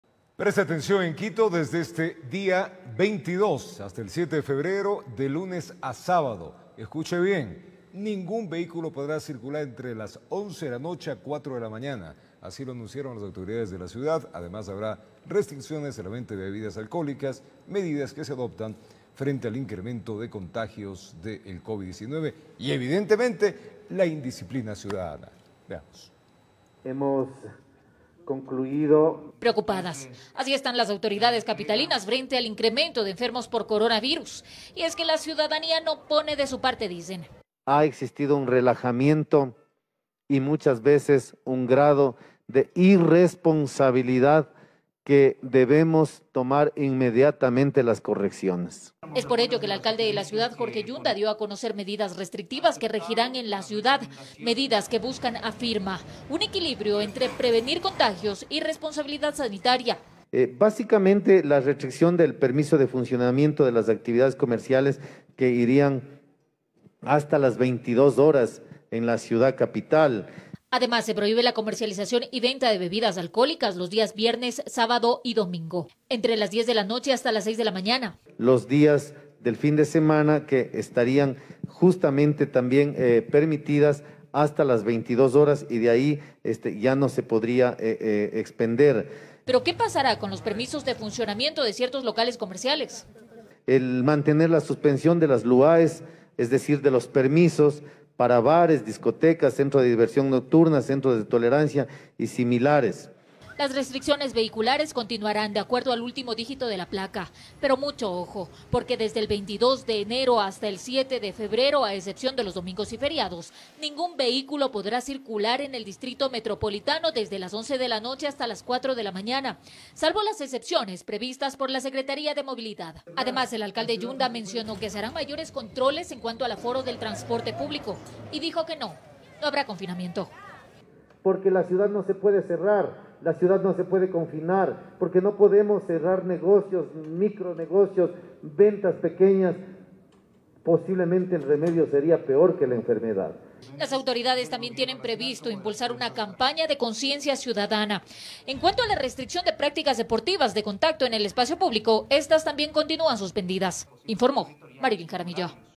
Quito adopta nuevas medidas restrictivas
El alcalde de Quito, Jorge Yunda, anunció nuevas medidas restrictivas en la ciudad para frenar el aumento progresivo de casos de contagios de Covid-19.